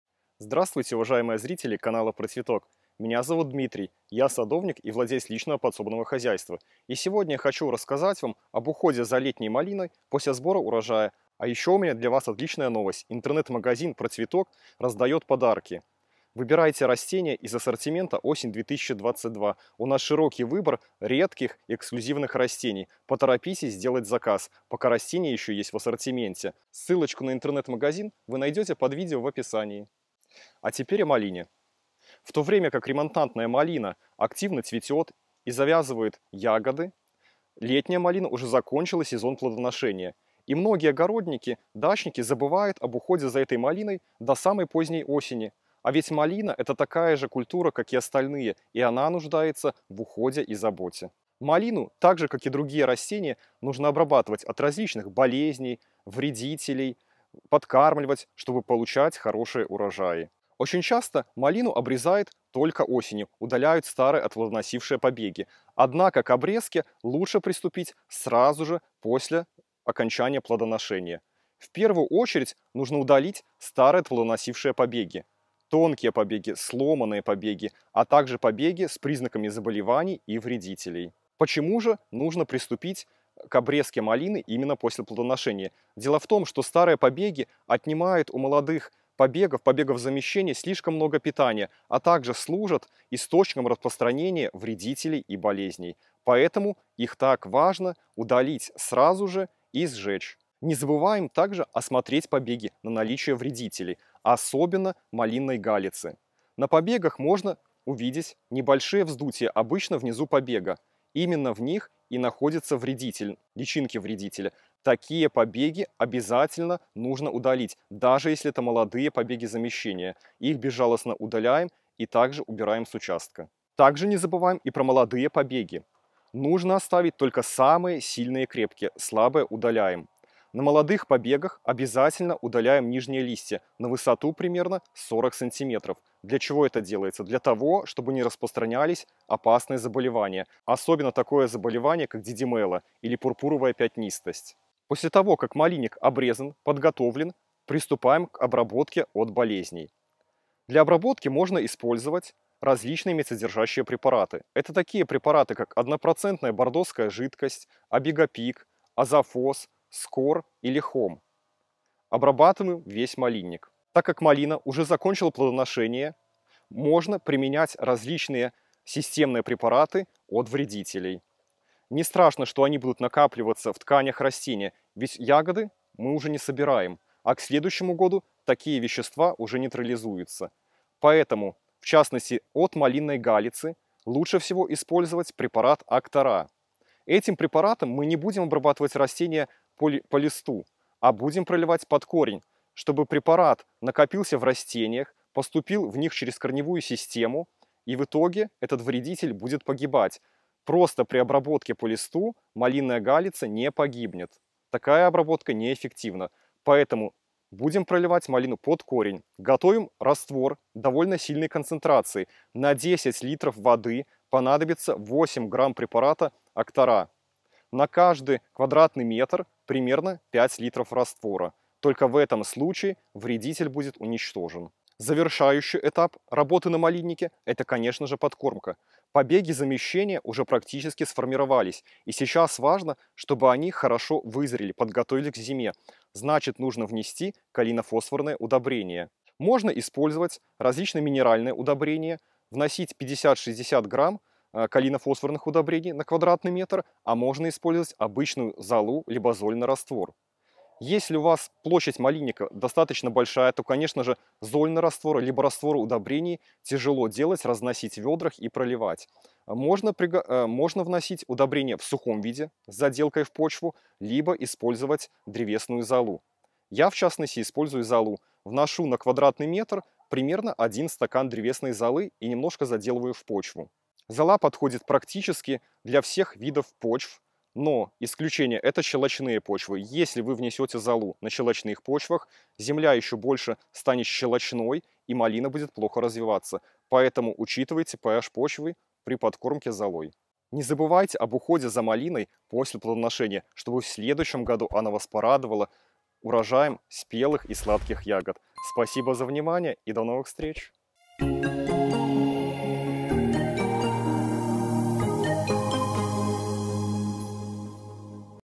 ОБЯЗАТЕЛЬНО СДЕЛАЙТЕ ЭТО ПОСЛЕ СБОРА УРОЖАЯ ЛЕТНЕЙ МАЛИНЫ: обработка, обрезка, подкормка
В то время как ремонтантная малина активно цветет и завязывает ягоды, летняя малина уже закончила свое плодоношение. Многие дачники забывают об уходе за кустами до поздней осени, а ведь летняя малина очень нуждается в уходе и заботе в это время. Что обязательно следует сделать с летней малиной после сбора урожая расскажет опытный садовод Дмитрий Кастрицкий.

00:00 Приветствие
00:12 Подарки от Procvetok
00:30 Уход за летней малиной после сбора урожая
01:00 Обрезка летней малины после сбора урожая
01:42 Малинная галица на побегах
02:05 Нормировка молодых побегов
02:28 Обработка малины от болезней
02:44 Обработка малины от вредителей
03:46 Подкормка летней малины